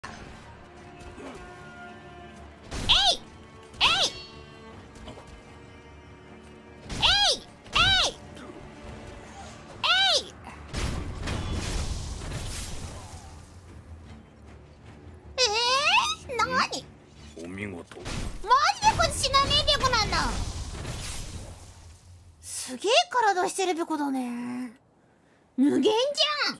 Pekora tries to kill NPC in tutorial, is surprised when she fails [Hololive/Eng Sub]
Maybe she's just surprised that the guy is essentially immortal...?

Apologies if my translations are incorrect-- I only know a bit of the language and tried to Google Translate/guess the more difficult parts.